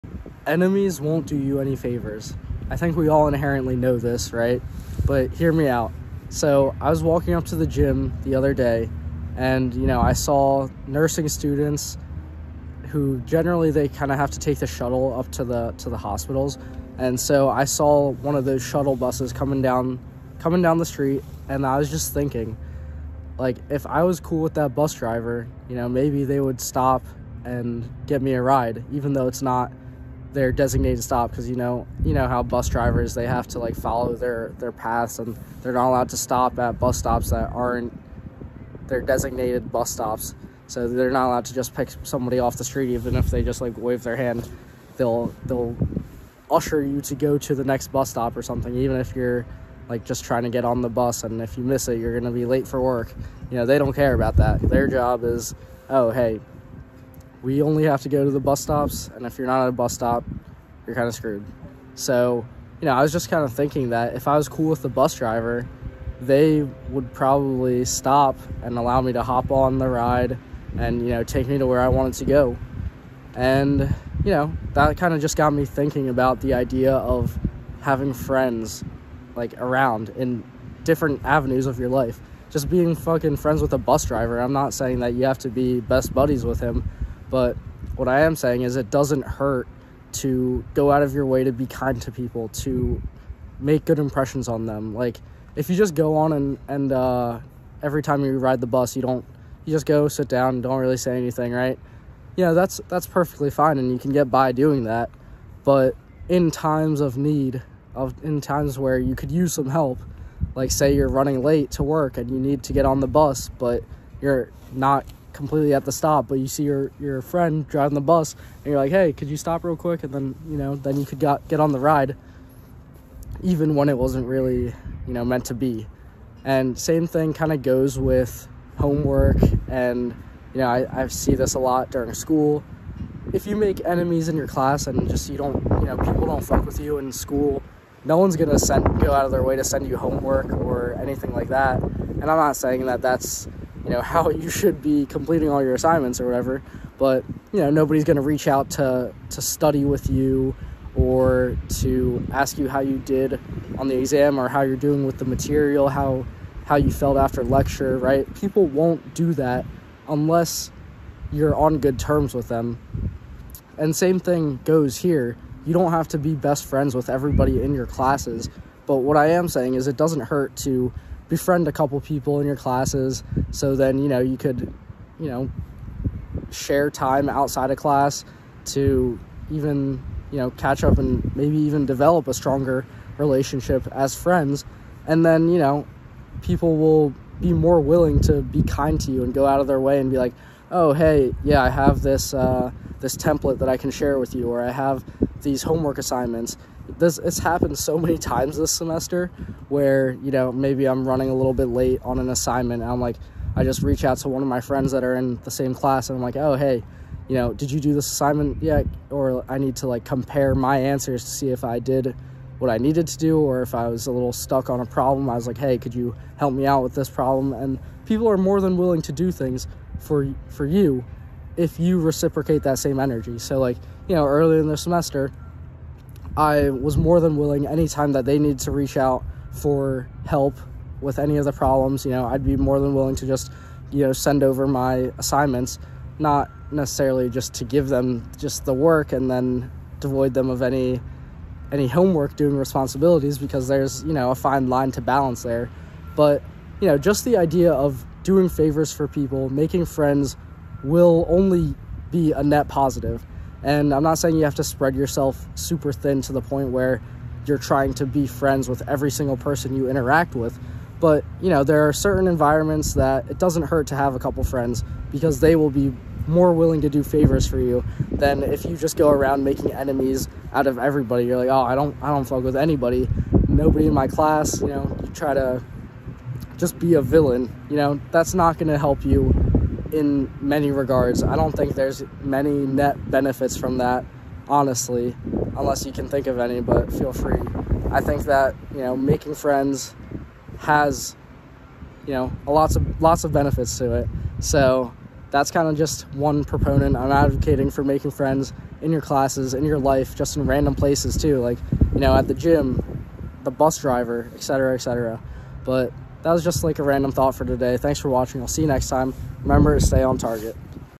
Enemies don't do you favors
In this video, I talk about a random thought I had while walking to the gym. Having a friend in different places in life won't hurt you. If anything, I see it as a net positive. For example, befriending the bus driver could grant you some special treatment that others might not receive. Another example would be in school. Friends are willing to share homework answers or study together for tests whereas making enemies won't do you any good.